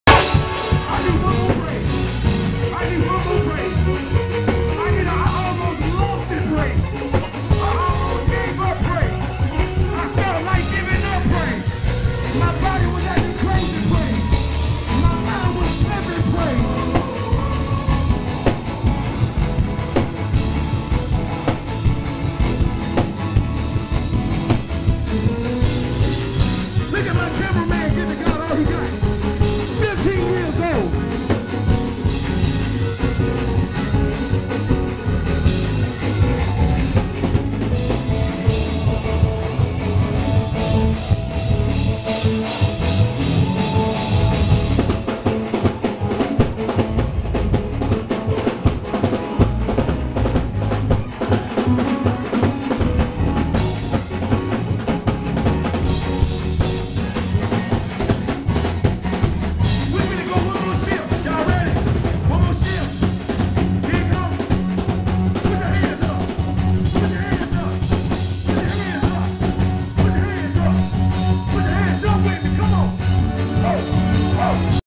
God Chasers Band Pt. 1
Band playing at God Chasers 2013